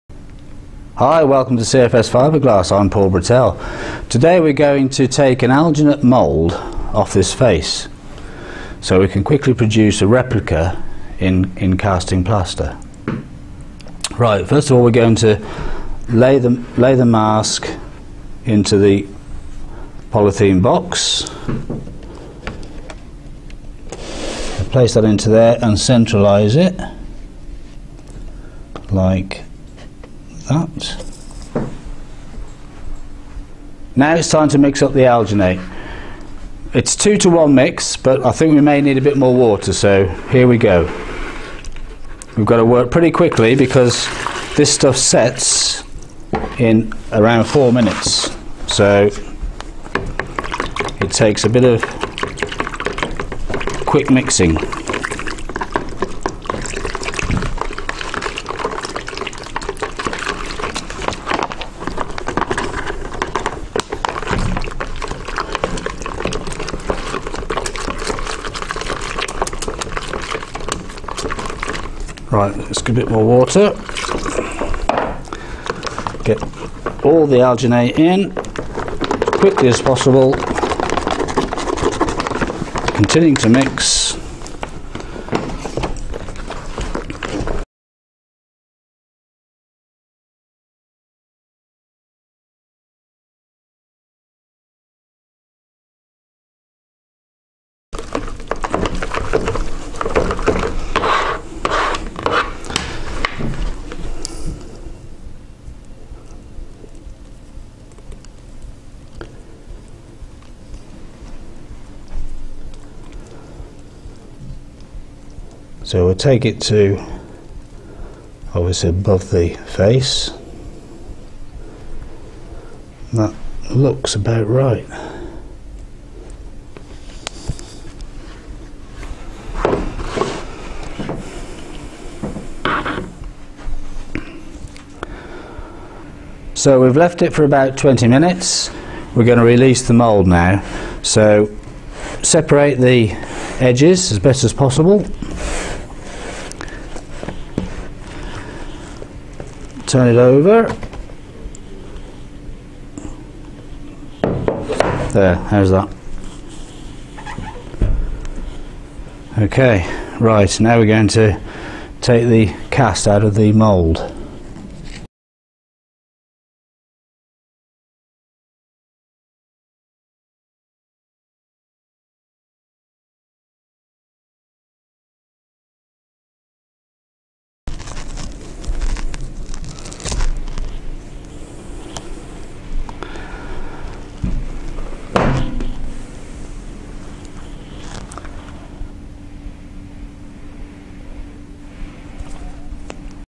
CFS Fibreglass Alginate Mould Making
CFS Fibreglass, basic use of alginate to make a mould of an object.